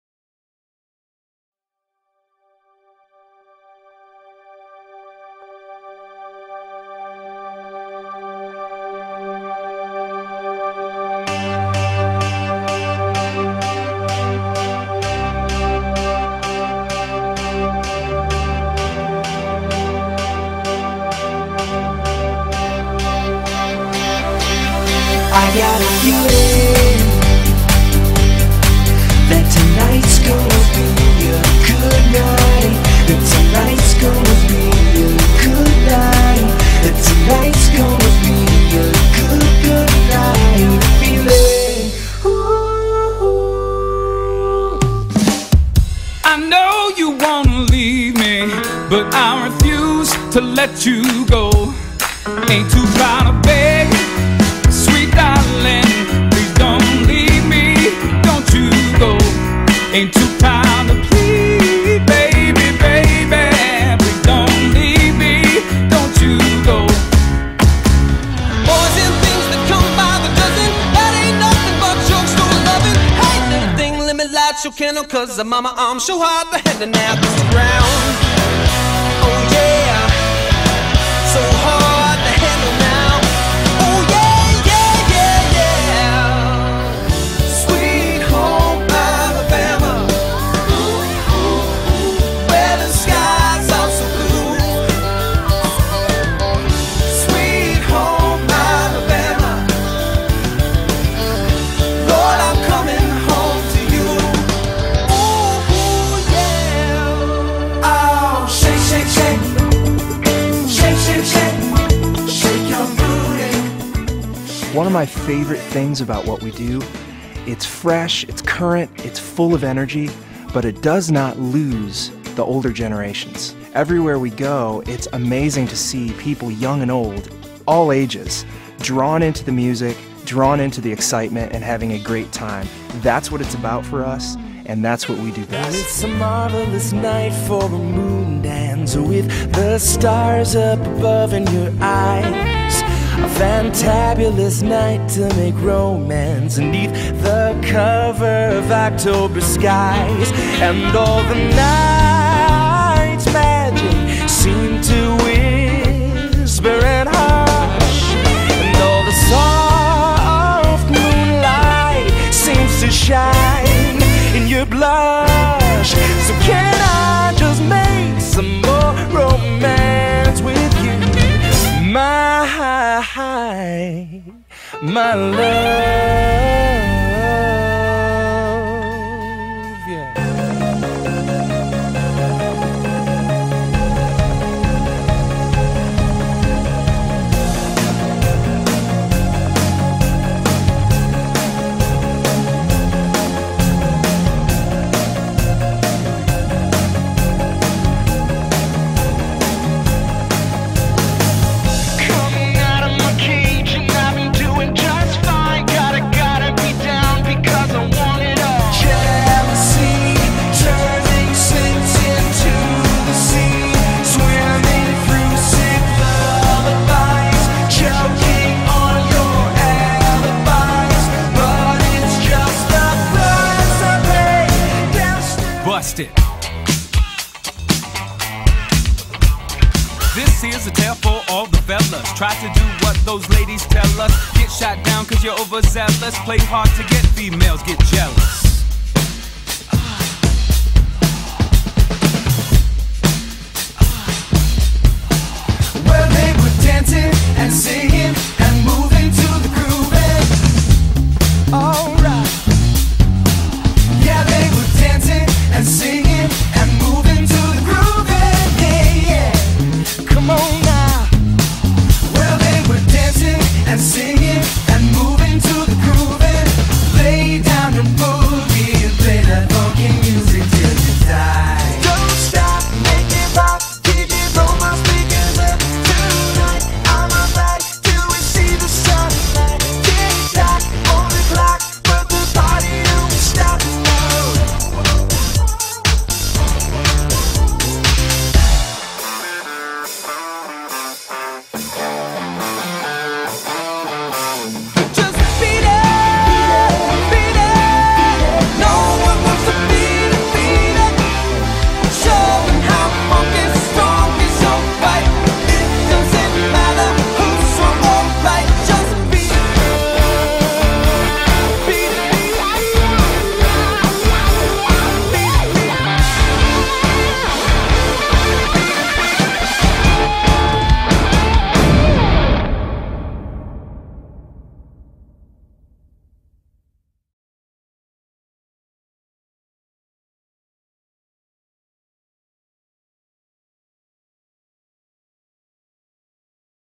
A Town A List Promotional Video
They are Atlanta's musical 'A-List'. A collection of accomplished artists who have toured the world with Bruce Springsteen, The Dave Matthews Band, The Black Eyed Peas, Neil Young, Jewel, The Killers, Stevie Wonder, Jennifer Lopez, and many more. They have combined forces to play for you, at your event!